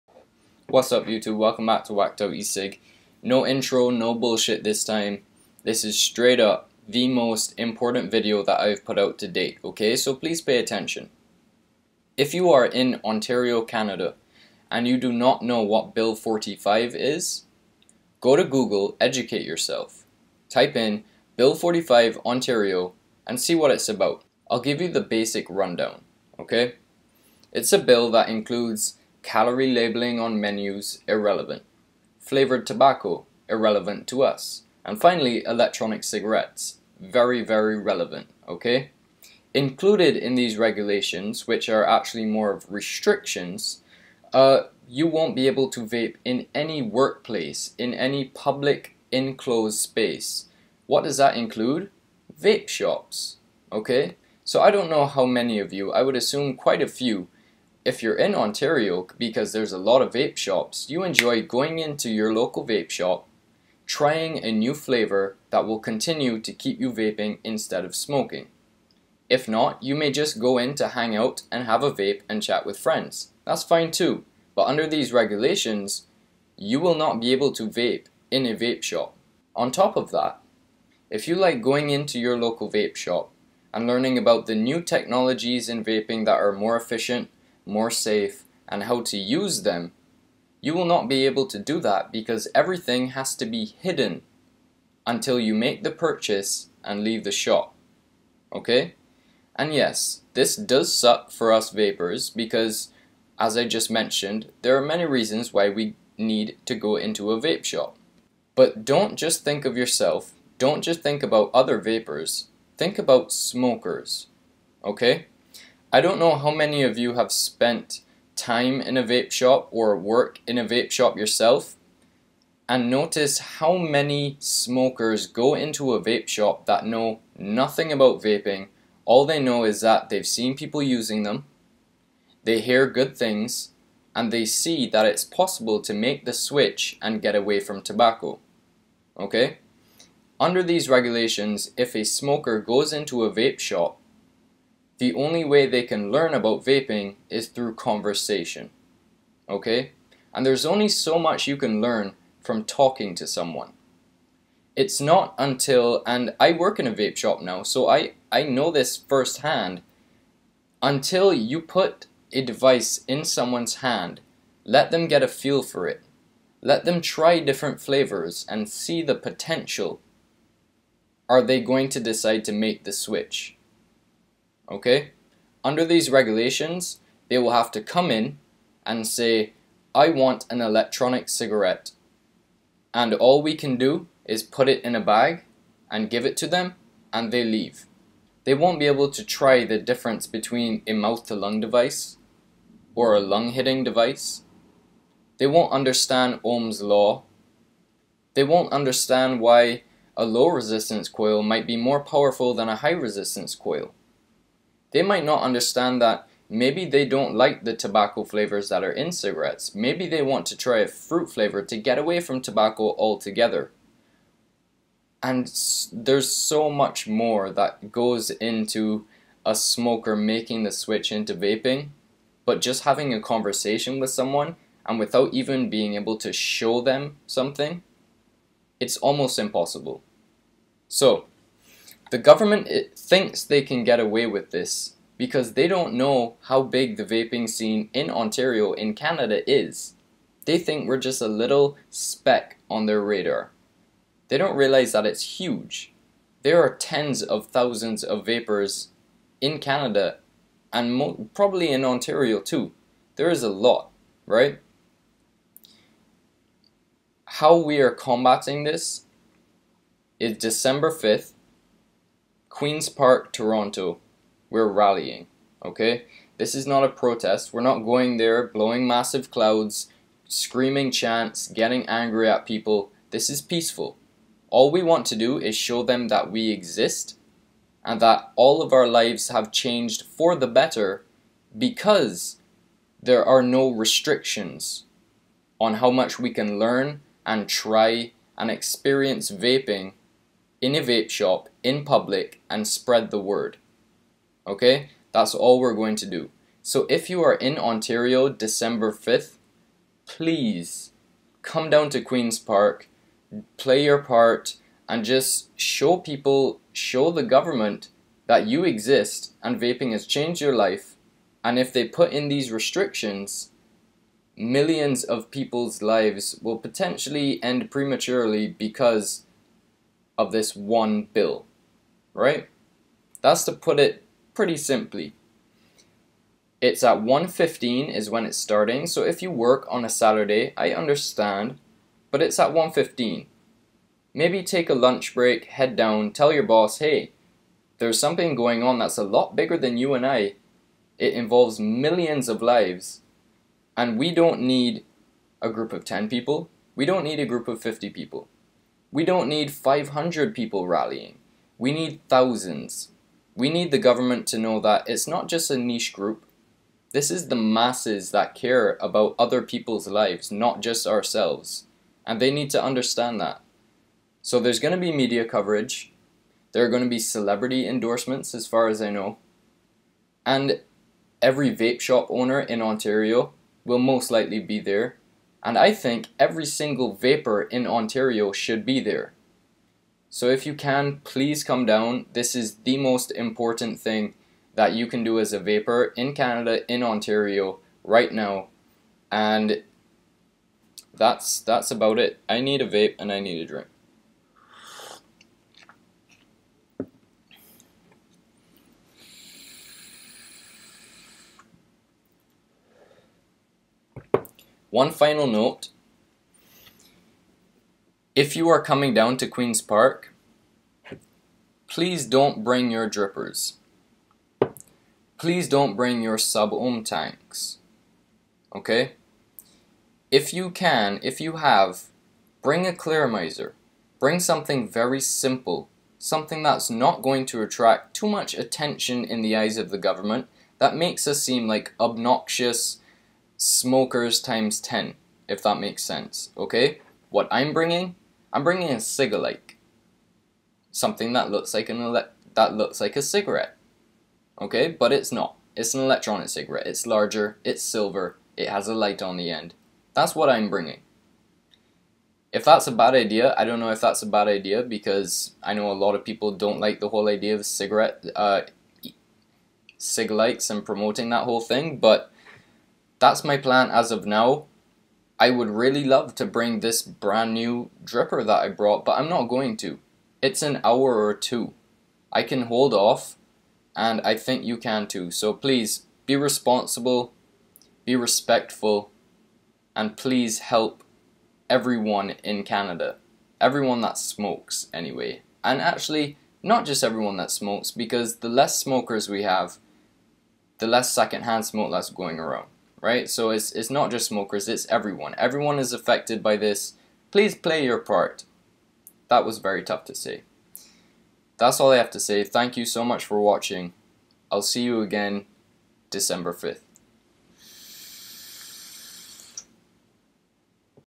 Ontario Call-To-Action *VERY IMPORTANT*
Please, if you are in Ontario December 5th; come to Queen's Park in Toronto. As a canadian vaper, this is the most important thing you can do right now to save vaping not only in Ontario, but Canada and potentially the rest of the world. This isn't small, we don't need a small group of people, we need THOUSANDS of people to show up and show the government and world how many peoples' lives vaping has saved so far and how many more can be transitioned away from Tobacco.

Stay updated on the Ontario Vapor Advocates facebook page for more information. Hope to see you there December 5th!

(bimvapor is my username)